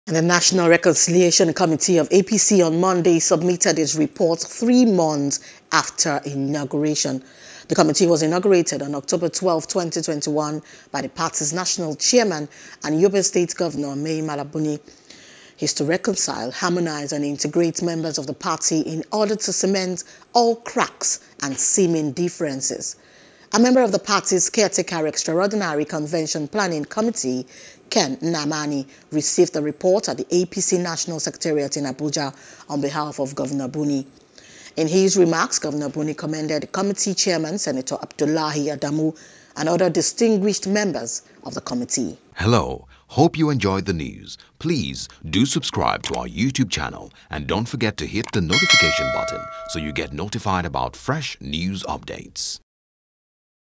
A.P.C.: National Reconciliation Committee Submits Interim Report | NEWS
The National Reconciliation Committee of APC on Monday submitted its report, three months after inauguration.